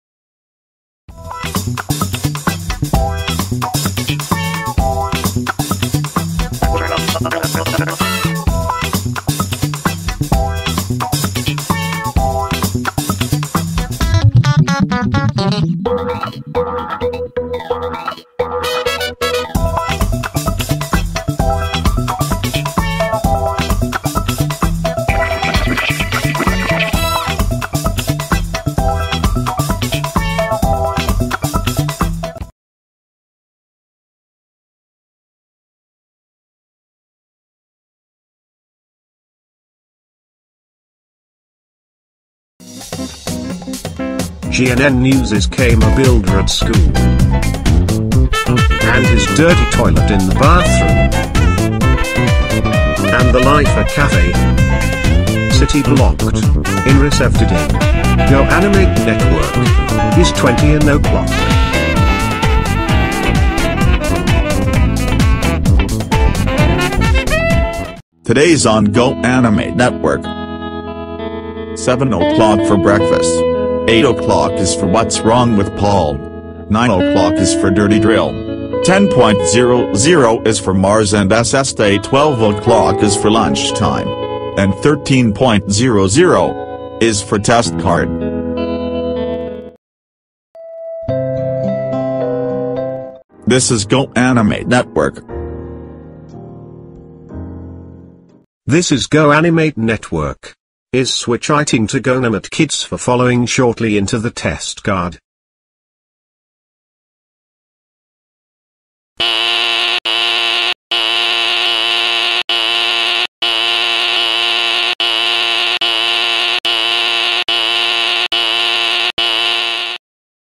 GoAnimate Network Sign Off 1997
1. Ending [Whats that noise]
2. GNN News
3. GoAnimate Network Old and New Logo
4. Brain says Testcard
5. GoAnimate Network Testcard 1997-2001